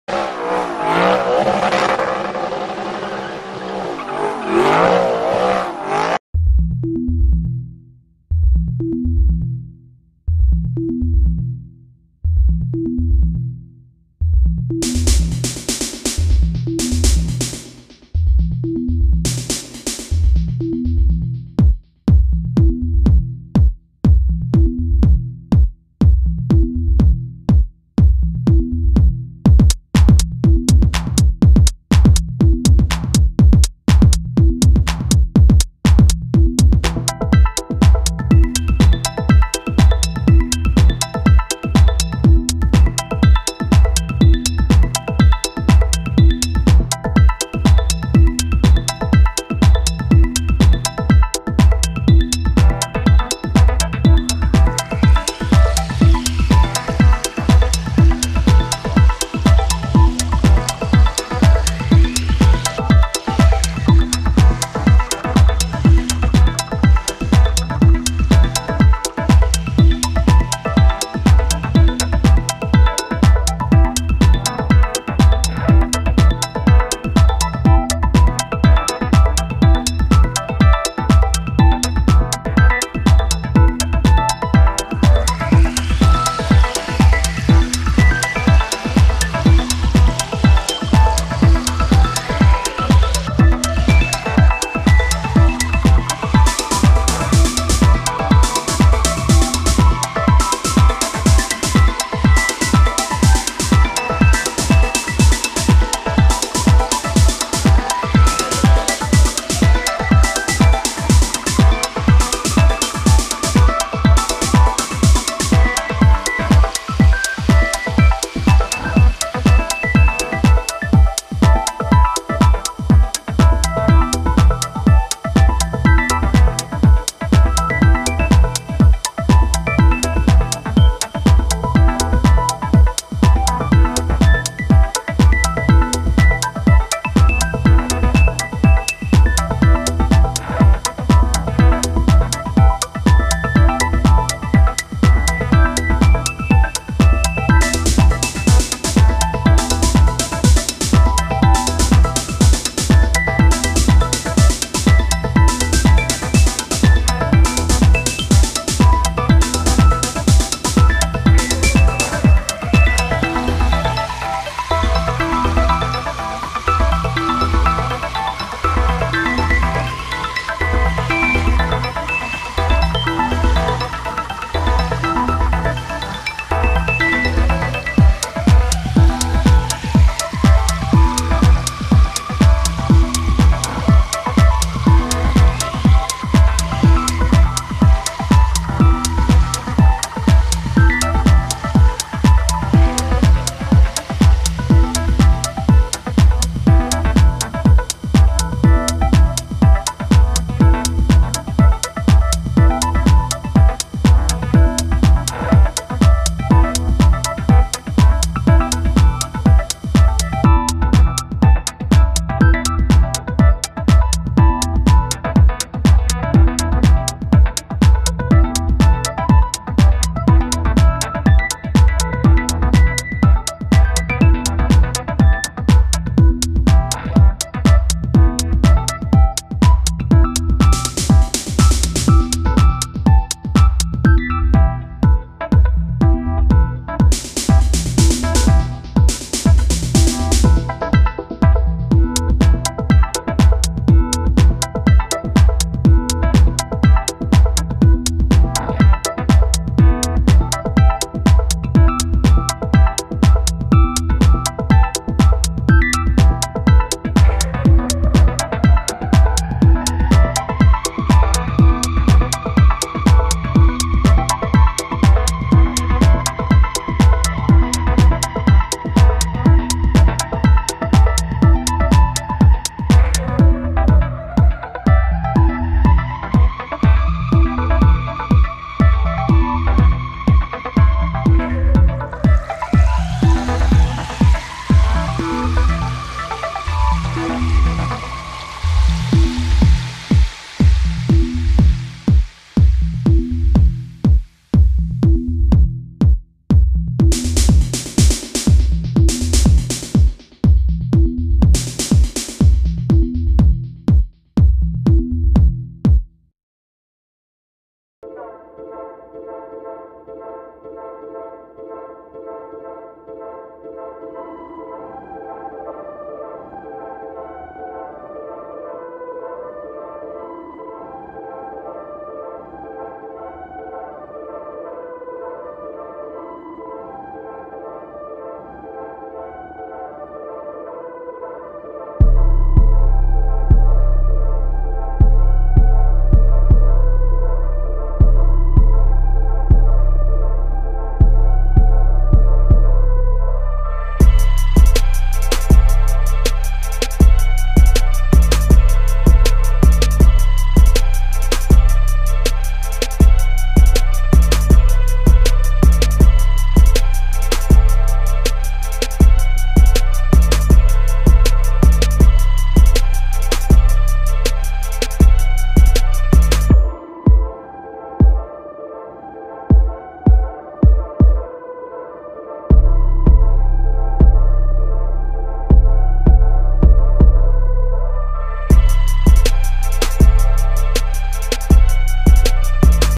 BMW 640d GT (Gran Turismo) 2018 Review (Interior & Exterior)
This video is in 1080p 🎥🎞📽🎬📷📸📹📼
Camera:Nikon
Price of the car: 87.987€
M/// Pack Exterior & Interior
360° Camera
Interior: Exclusive Nappa Leather Schwarz⚫
Car Colour: Carbonschwarz